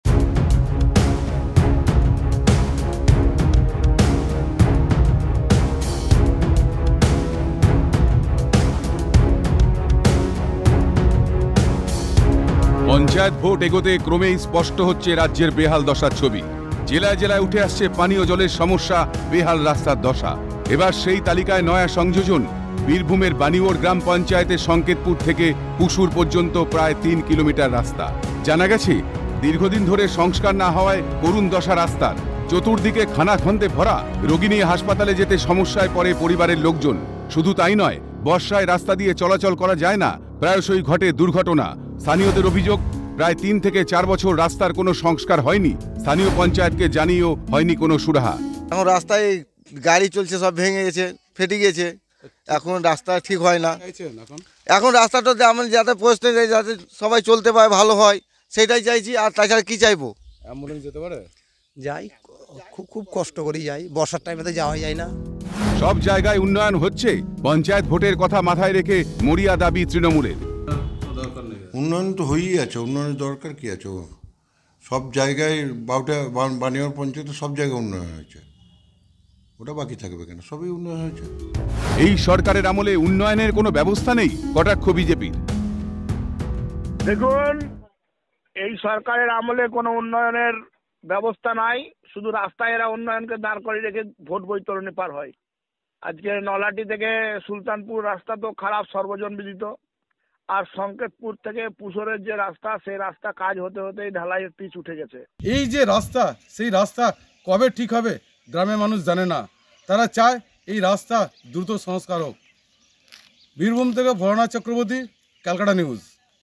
রাস্তার হাল বেহাল, নজর নেই প্রশাসনের | CN
ভোট আসে, ভোট যায়। রাস্তার কোনও সংস্কার হয় না। দিনের পর দিন বেহাল অবস্থায় পড়ে রয়েছে রাস্তা। ভোগান্তিতে স্থানীয়রা। ছবিটা, বীরভূমে বাণীওর গ্রাম পঞ্চায়েতের।